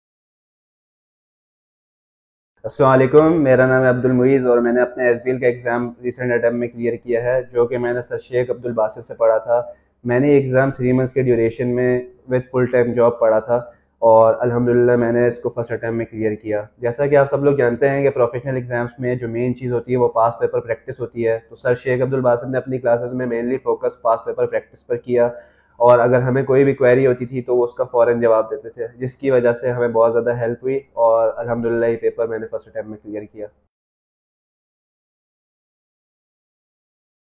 A Journey of Success: Meet Moiz, an SBL Student of Sir Sheikh Abdul Basit
In this inspiring testimonial video, Moiz shares his incredible achievement as he successfully cleared the SBL (Strategic Business Leader) exam on his very first attempt under the guidance of renowned educator, Sir Sheikh Abdul Basit.